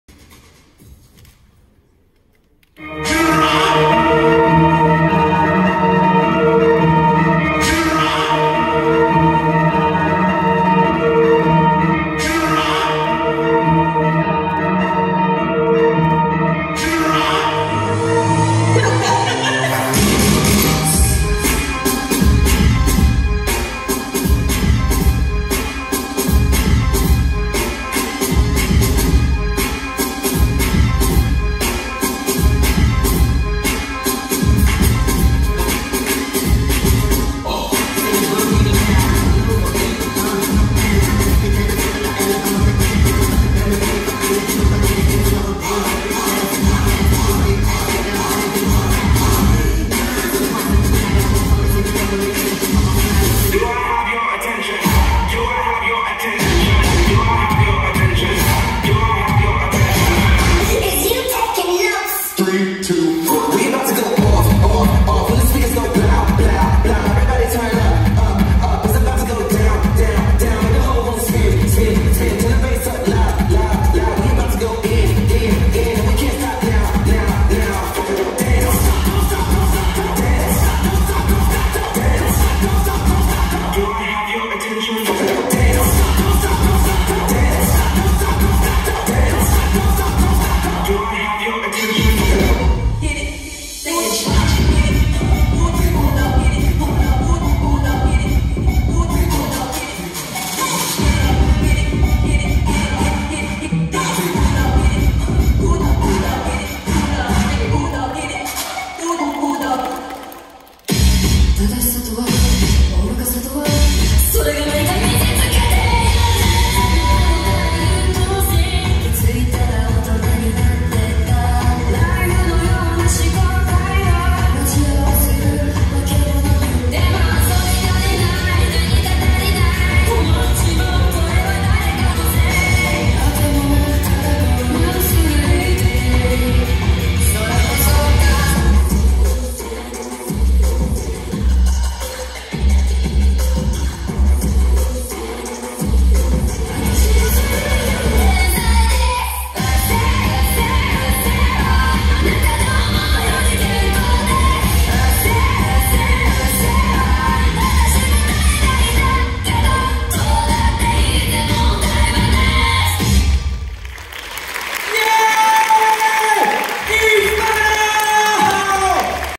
E-SPIDER【DDDE2021/OPEN部門】
Double Dutch Delight East 2021（ダブルダッチデライトイースト）
イースパイダー
順位：16位

【得点】68点
技術力：13
表現力：13
構成力：14
オリジナリティ：14
完成度：14